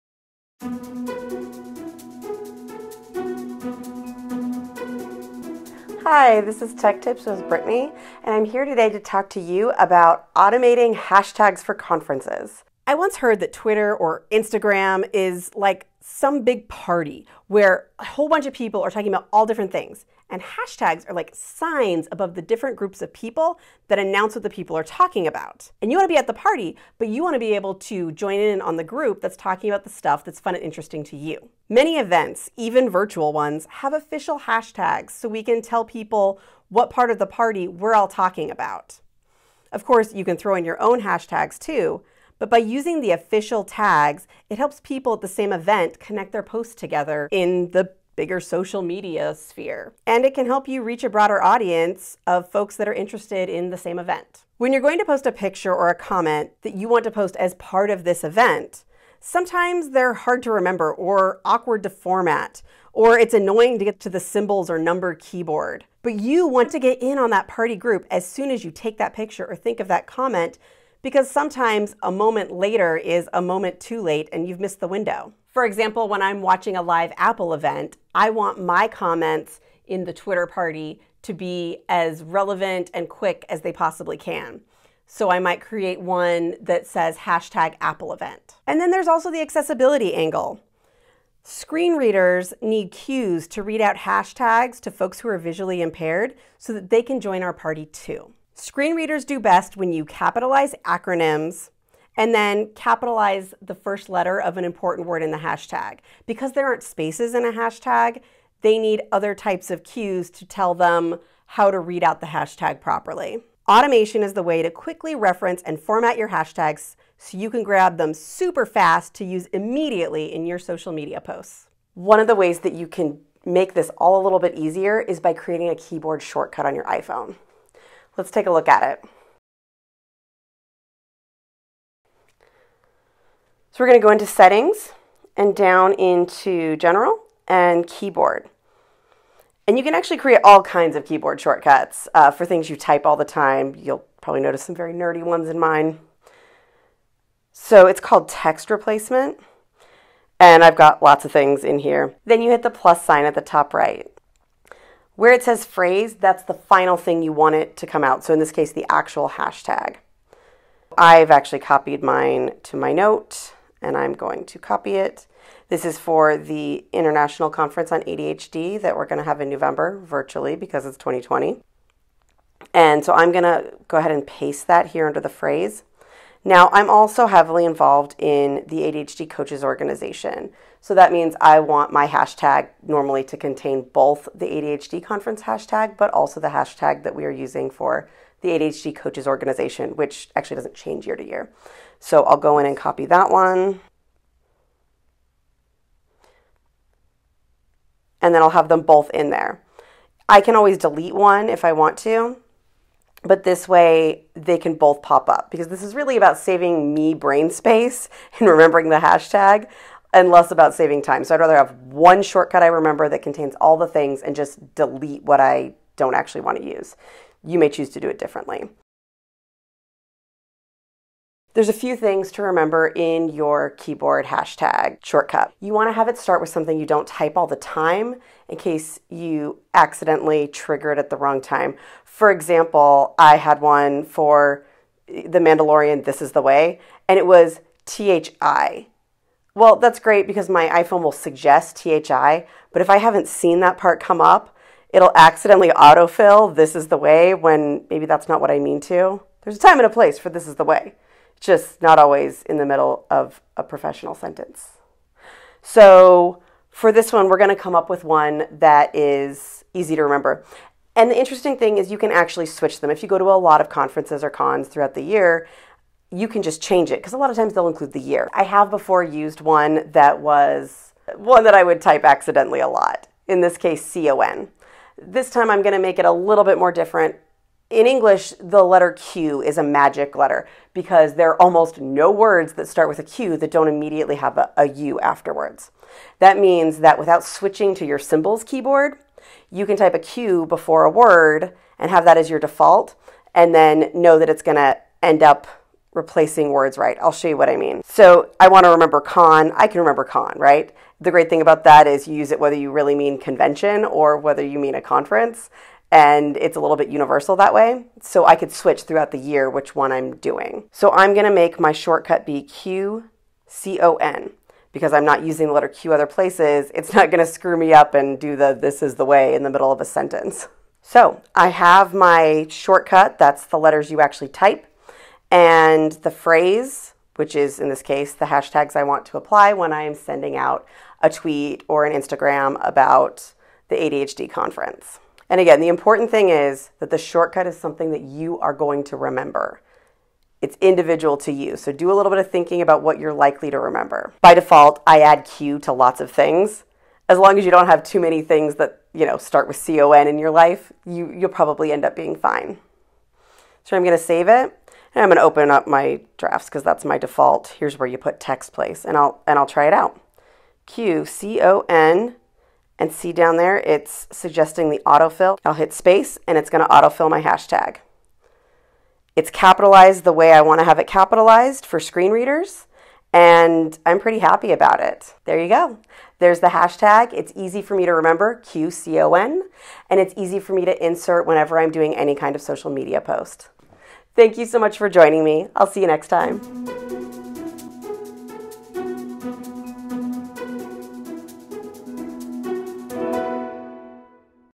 Automate Hashtags for Conferences and Cons on your iPhone
Brittany shows you how to remember all those Hashtags from conferences and conventions you attend when you post to your social medias.

Have you ever been in the middle of a conference or convention and come across a great picture or video you want to post to social media to share with the world, but you can’t remember the hashtag for the event and say to yourself you will post it later, but then forget? One of the funnest parts of posting on social media is letting everyone know where you are and what events you maybe attending, but if you can’t remember the hashtags for the events your post may not reach as many people as you’d like.  Brittany shows you a clever way to keep those hashtags at your fingertips in a moments notice so you can make your posts in the moment instead of waiting and possibly forgetting to share your amazing posts later.  Use this keyboard shortcut and don’t deprive the world of your awe-inspiring moments.

Intro
You’re at a con or a conference— the sights, all the exciting things to see or people to meet. Everything is a whirlwind. Unless you have a memory of steel and thumbs of lightning, it’s easy to miss posting the hashtag with your photo so folks can find it. I mean, that’s the point, right? Show off your story or draw attention to your professional profile or organization. But if the hashtag isn’t there, it’s easy to lose in the Internet curfuffle. Or to mistype something and get it wrong.

Be kind, format your hashtag. It’s easier to see. Besides, it’s courteous to format them well. Screenreaders for folks with print disabilities or visual impairments are better at pronouncing.

Put in the hashtag you want, come up with a great keyboard shortcut, and you’re ready to post to all the socials!

Tech Tips with Brittany Smith is hosted by Devise & Conquer Coaching which offers Technology, Productivity, and ADHD Coaching. Music by Brian Freeland of B. Free Productions. Edited by Derek Miley.